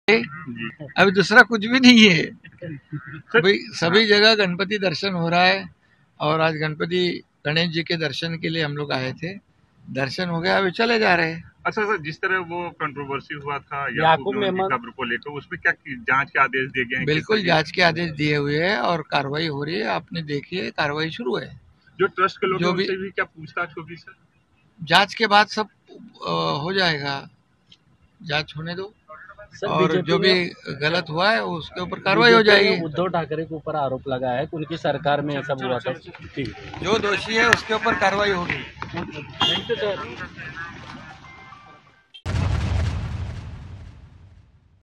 Yakub Memon की कब्र को लेकर CM Eknath Shinde ने दिया बड़ा बयान | Maharashtra | Aaj Tak | Latest News
गुरुवार रात करीब पौने 12 बजे महाराष्ट्र के मुख्यमंत्री एकनाथ शिंदे राज्य के उपमुख्यमंत्री देवेंद्र फडणवीस के घर गणपति बप्पा के दर्शन के लिए पहुंचे